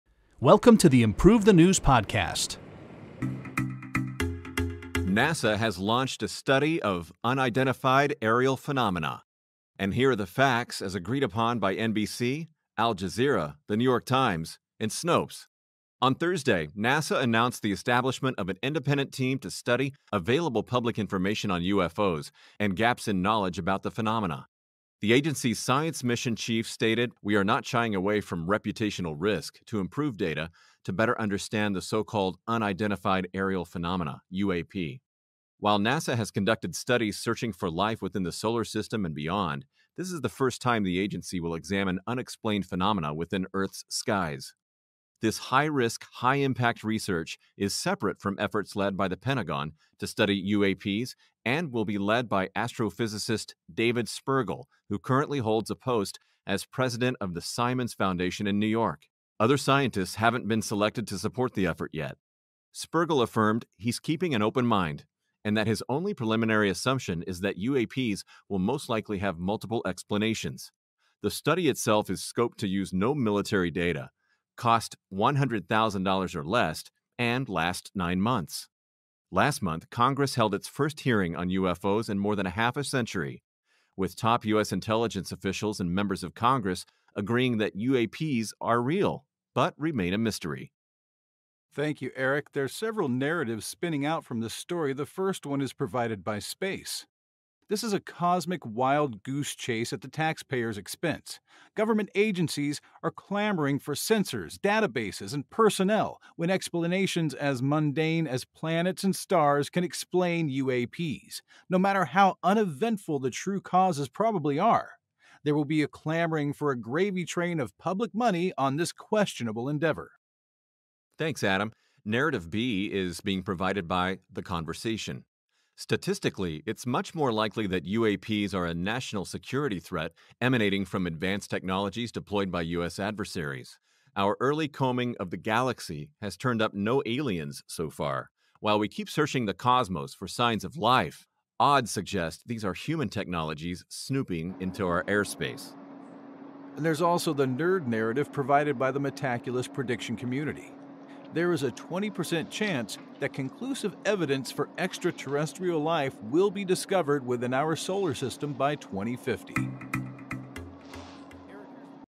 NASA Launches Study of UFOs
NASA announced the establishment of an independent team to study available public information on UFOs and gaps in knowledge about the phenomena. The agency's science mission chief stated "We are not shying away from reputational risk" to improve data to better understand the so-called Unidentified Aerial Phenomena (UAP).